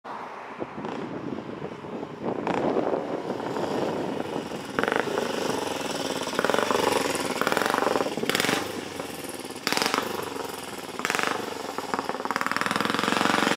Norton Manx Racer 1937 m40 International wales test
Norton International road test Harold Daniell 1937n 350cc ohc junior tt